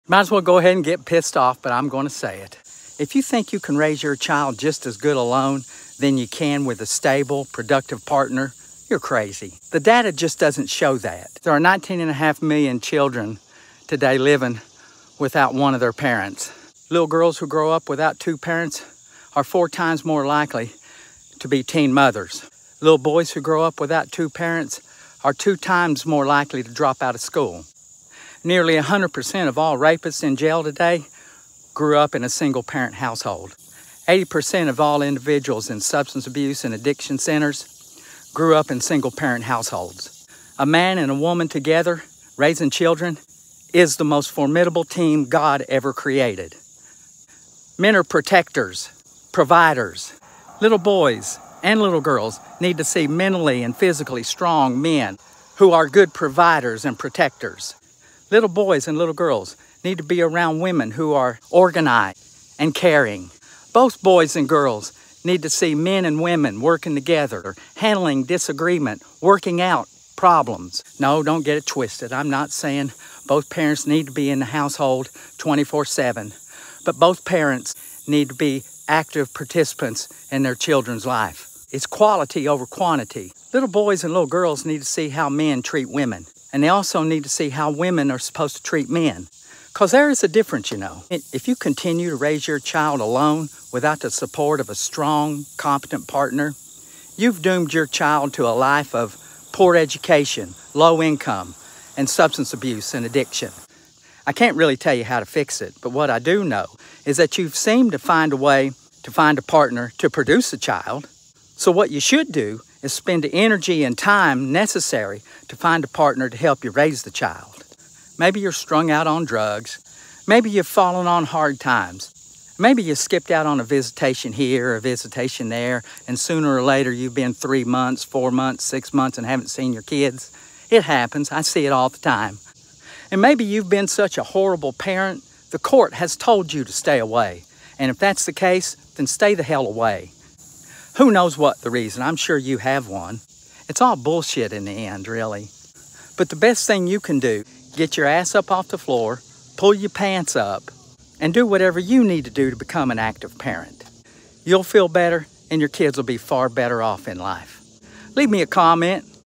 Why you need a partner to raise your children
Single parent children don’t fair well. Why single parents shouldn’t raise kids. Stop drug addiction before it starts